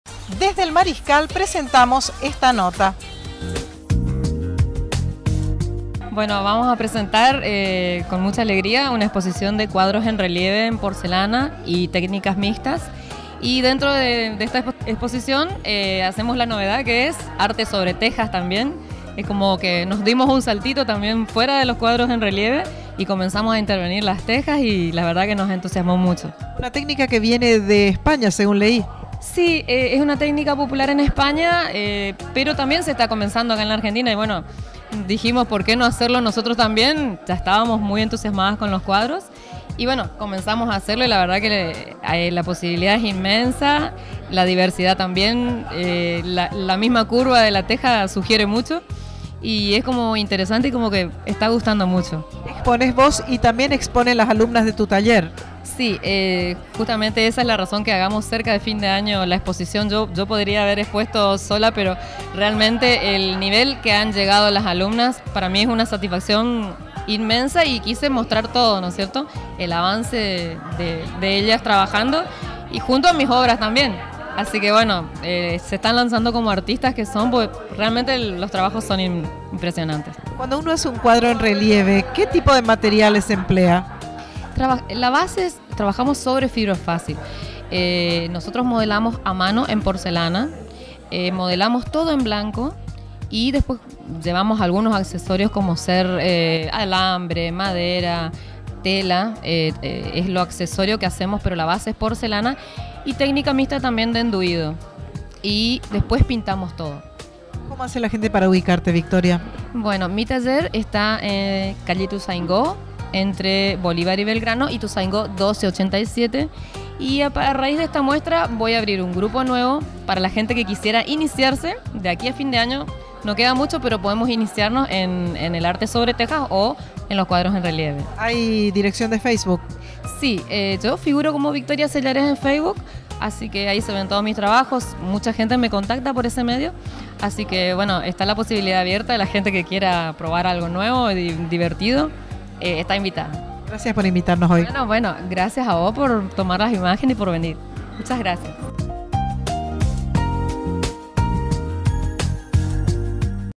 Victoria Sellares en el Mariscal
Victoria y sus alumnas/os presentaron trabajos en una muestra que se lleva a cabo en el Café Literario El Mariscal . .